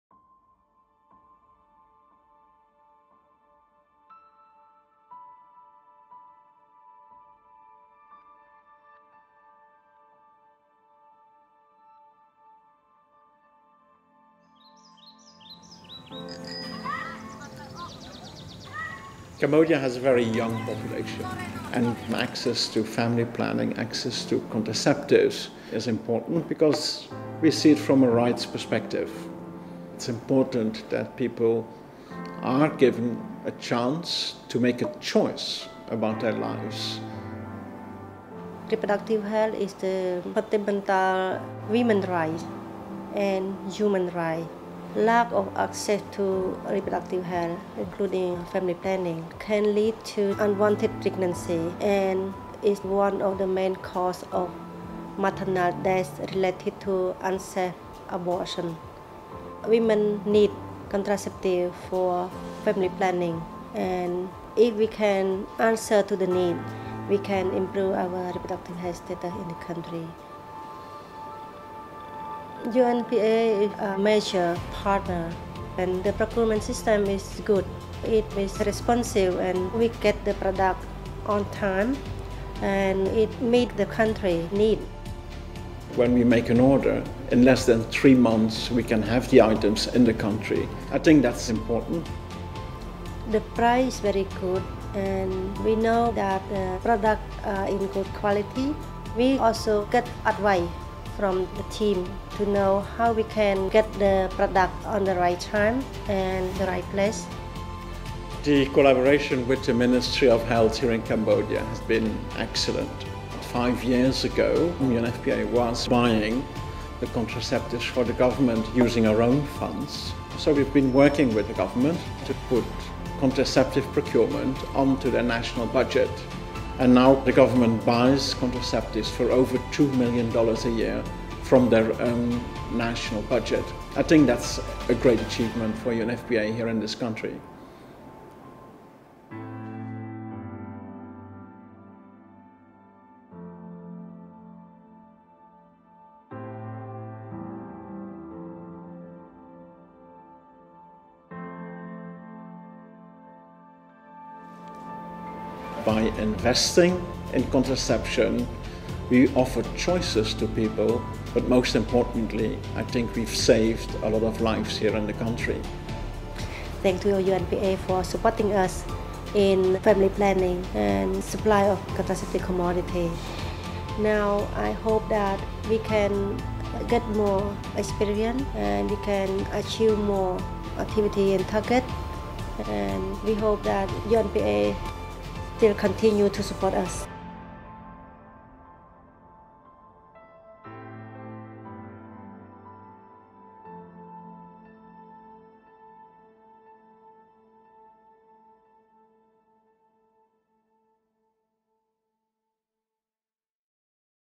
Planejamento familiar no Camboja: acesso a saúde sexual e reprodutiva
O Fundo de População das Nações Unidas (UNFPA) e parceiros locais ajudaram o governo do Camboja a adquirir suprimentos anticoncepcionais durante anos. Após uma decisão histórica, o governo estabelece uma linha orçamental nacional para a aquisição de contraceptivos. e